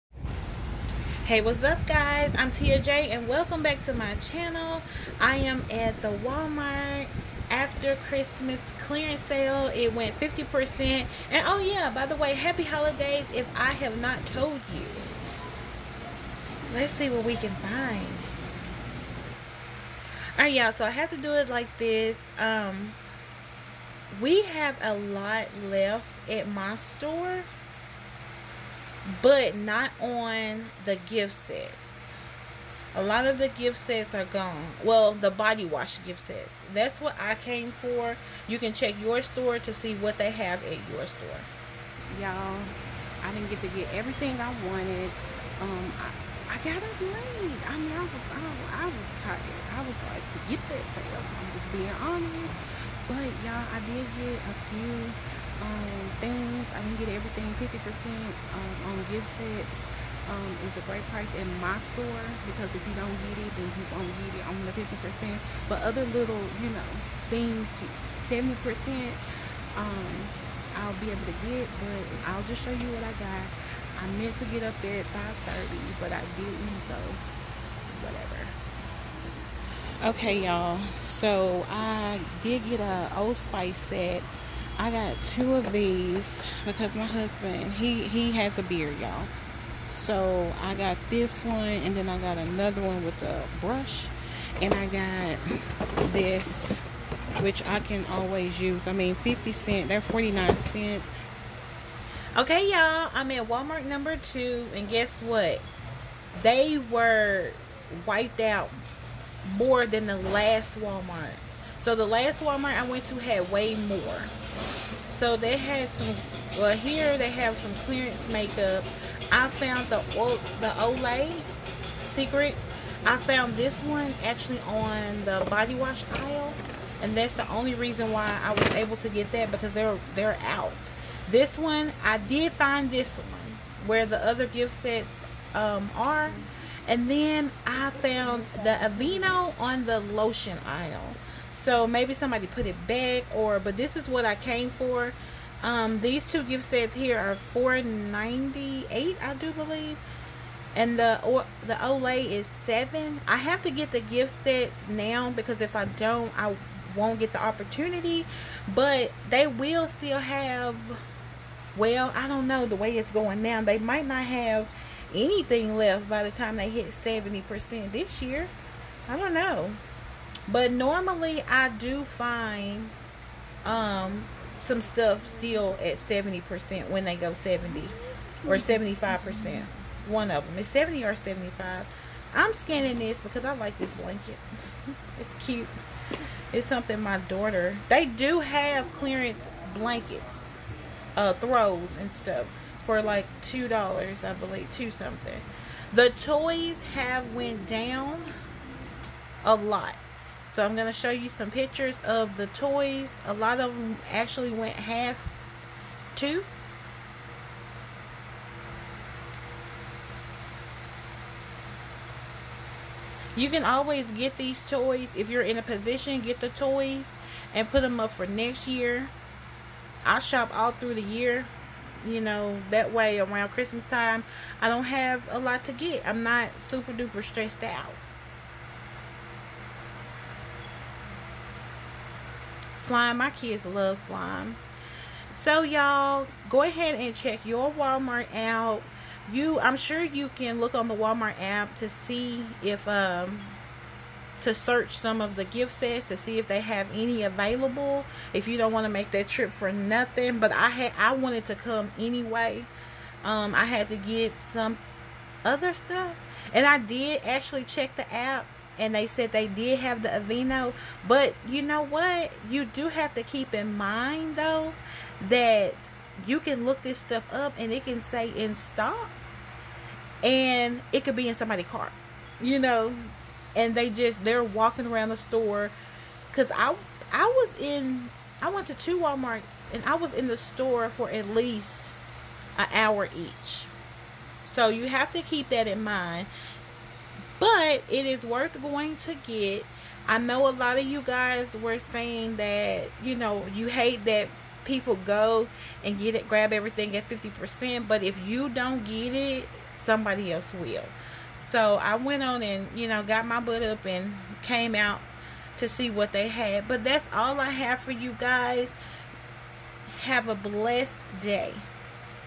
Run❗❗WALMART AFTER CHRISTMAS CLEARANCE DEALS/2 STORES CHEAP ITEMS
I look forward to the after Christmas sales every year because that is one way to save money on items you need. It is 50%off right now.